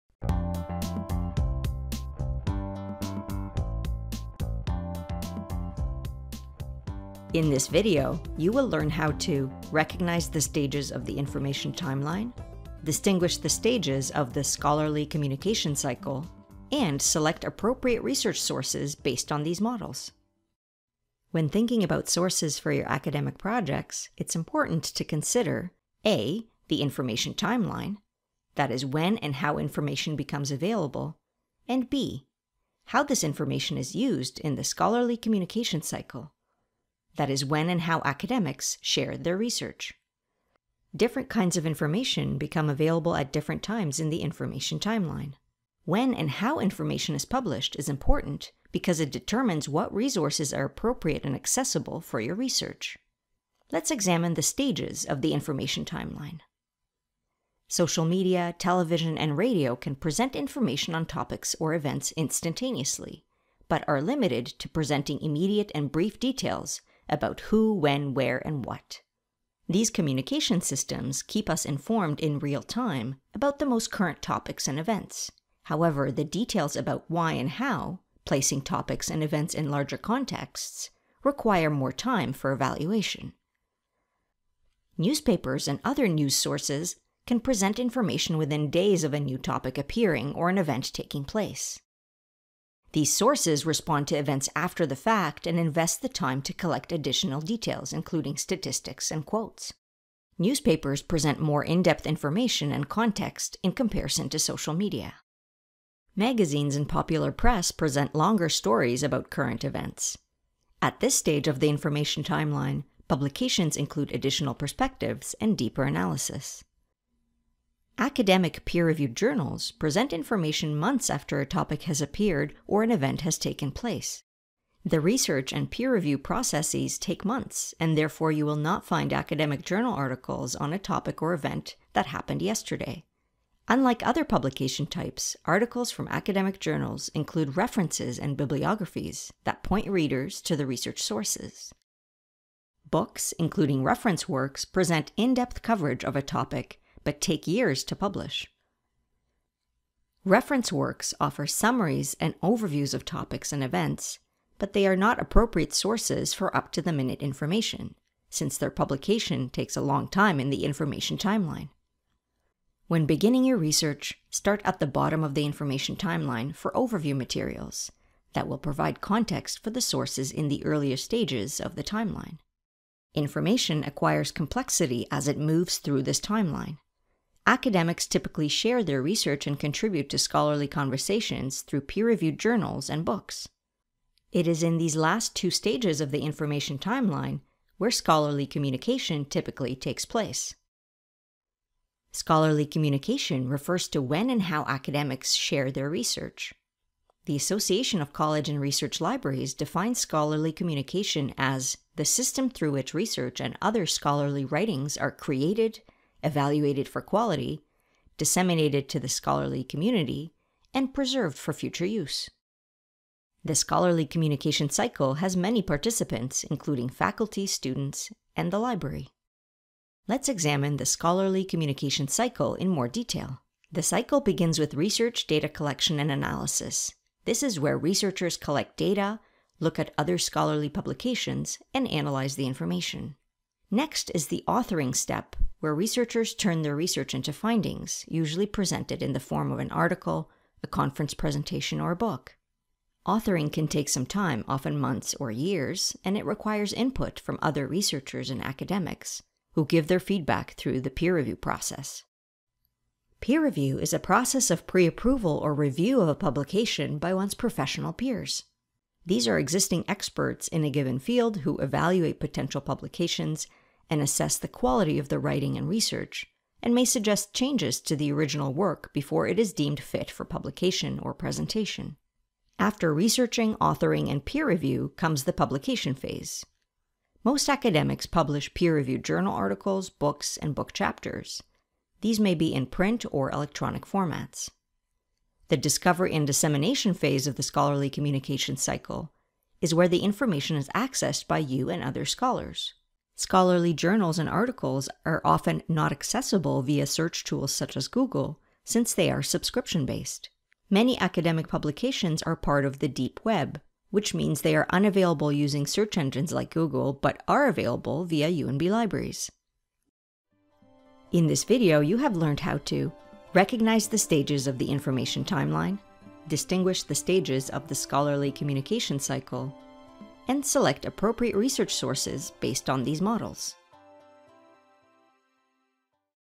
Information Timeline and Scholarly Communication Cycle
In this video, you will learn how to recognize the stages of the information timeline, distinguish the stages of the scholarly communication cycle, and select appropriate research sources based on these models.